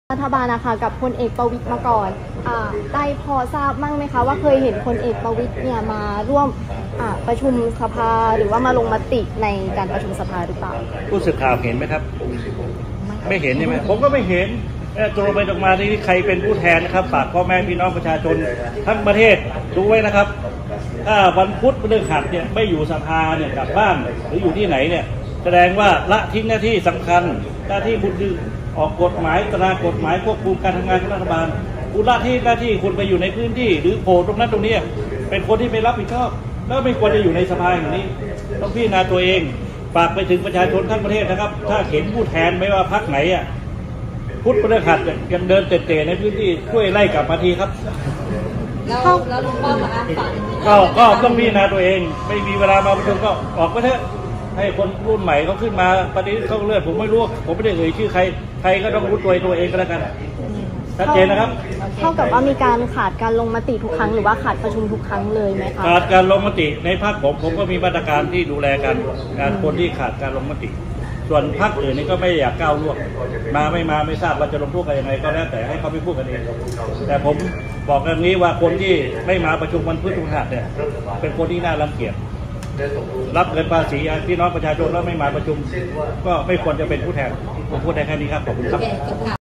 “วิสุทธิ์” ซัดน่ารังเกียจไม่ควรเป็นส.ส.หนีประชุมลงพื้นที่ฝากปชช.ไล่ จี้ "ลุงป้อม" ลาออกไม่เคยโผล่หัว
ปธ.วิปรัฐบาล ซัด ส.ส.ลงพื้นที่หนีประชุม เป็นคนน่ารังเกียจ ไม่ควรเป็นผู้แทน ฝากปชช.หากเจอให้ไล่กลับสภา ยัน ไม่เคยเห็น “บิ๊กป้อม” มาประชุมสักครั้ง ลั่น “ลาออกไปเถอะ”